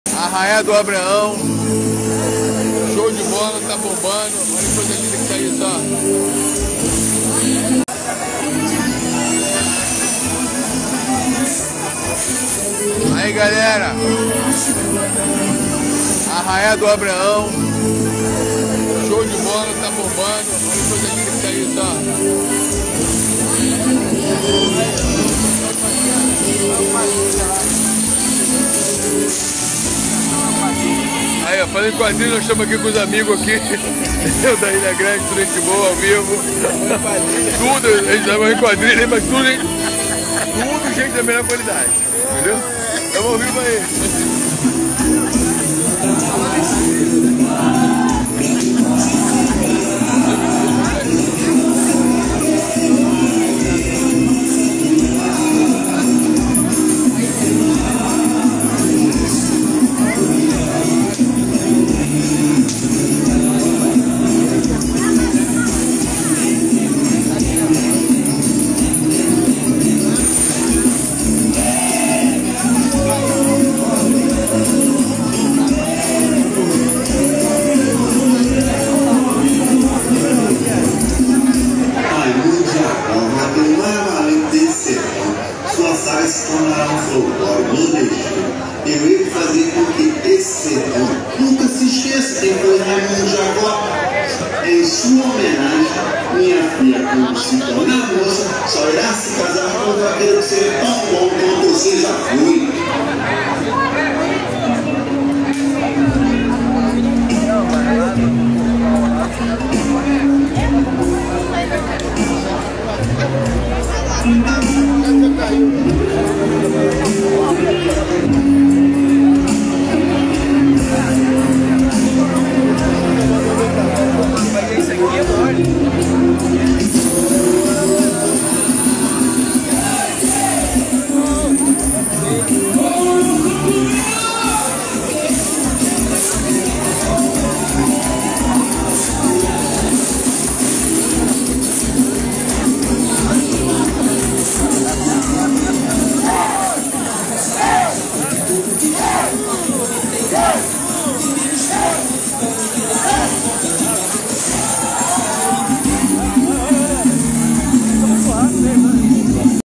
ARRAIÁ DO ABRAÃO - ILHA GRANDE! LOTADO!
Primeiro dia do arraiá do Abraão na Ilha Grande! Lotado!
Vídeo fonte: Nelson Ilha.